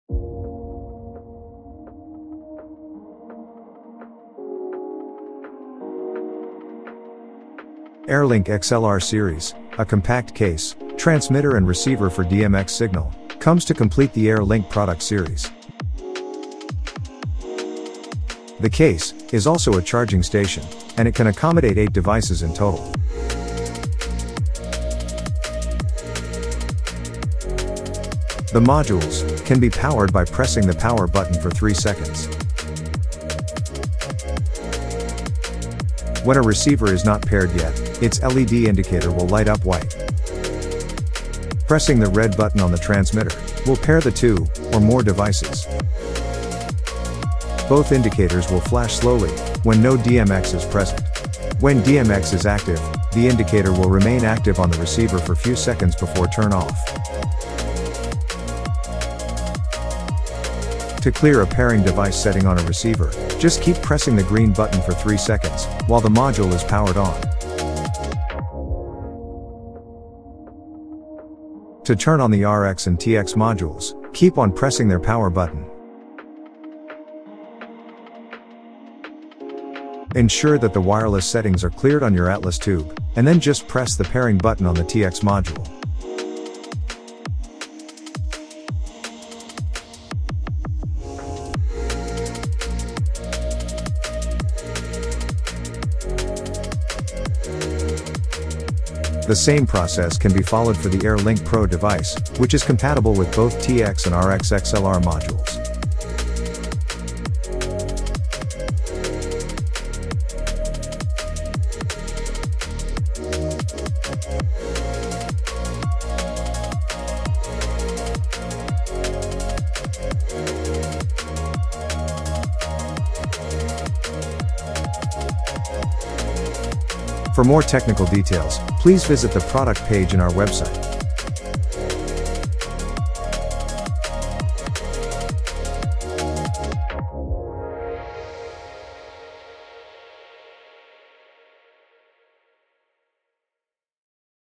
AirLink XLR Series | Battery DMX Transmitter & Receiver modules
Battery-operated wireless DMX transmitter and receiver, compatible with the AirLink series, the Luminus, and Atlas Tubes. The series includes an optional accessory, a case, and a charging station for 8 modules.

Each module can be charged by USB-C cable as well, which is included in the packing.